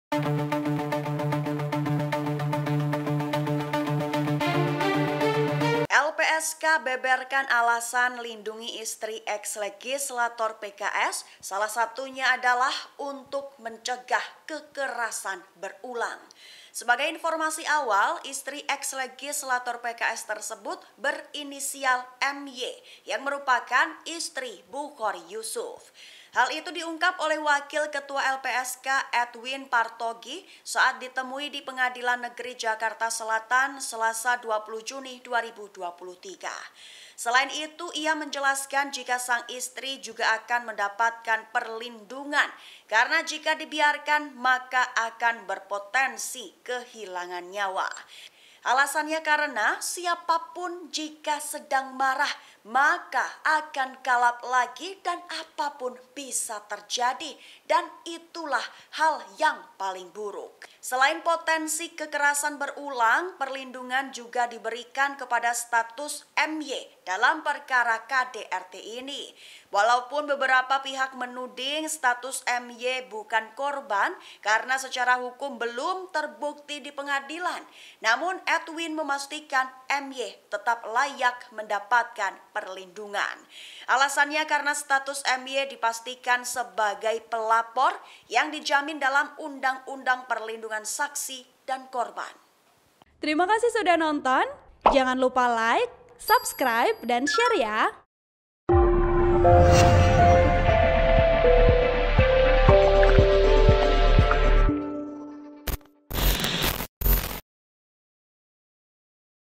Perkara KDRT Istri Bukhori Yusuf Berpotensi Kehilangan Nyawa! LPSK Beri Alasan MY Harus Dilindungi
TRIBUN-VIDEO.COM - LPSK beberkan alasan lindungi istri Eks Legislator PKS, salah satunya adalah untuk mencegah kekerasan berulang.

Sebagai informasi awal, istri Eks Legislator PKS tersebut berinisial MY yang merupakan istri Bukhori Yusuf.

Hal itu diungkap oleh Wakil Ketua LPSK, Edwin Partogi saat ditemui di Pengadilan Negeri Jakarta Selatan, Selasa (20/6/2023).

"Tentu kita mengantisipasi agar kekerasan yang dilakukan itu tidak terjadi kembali," ujar Wakil Ketua LPSK, Edwin Partogi saat ditemui di Pengadilan Negeri Jakarta Selatan, Selasa (20/6/2023).

Selain itu, ia menjelaskan jika sang istri juga akan mendapatkan perlindungan, karena jika dibiarkan, maka akan berpotensi kehilangan nyawa.

Alasannya karena, siapapun jika sedang marah, maka akan kalap lagi dan apapun bisa terjadi, dan itu hal yang terburuk.

"Kalau dibiarkan memang punya potensi (kehilangan nyawa). Kalau orang lagi marah, lagi kalap, apapun kan bisa terjadi, yang terburuk," kata Edwin.

Selain potensi kekerasan berulang, perlindungan juga diberikan karena status MY dalam perkara KDRT ini.

Walaupun beberapa pihak menuding status MY bukan korban karena secara hukum belum terbukti di pengadilan, namun Edwin memastikan MY tetap layak mendapat perlindungan.

Alasannya karena status MY dipastikan sebagai pelapor yang dijamin dalam Undang-Undang Perlindungan Saksi dan Korban.

(Tribun-Video.com/YessyWienata)

Artikel ini telah tayang di Tribunnews.com dengan judul LPSK Beberkan Alasan Lindungi Istri Eks Legislator PKS: Cegah Kekerasan Berulang,
Penulis: Ashri Fadilla
Editor: Erik S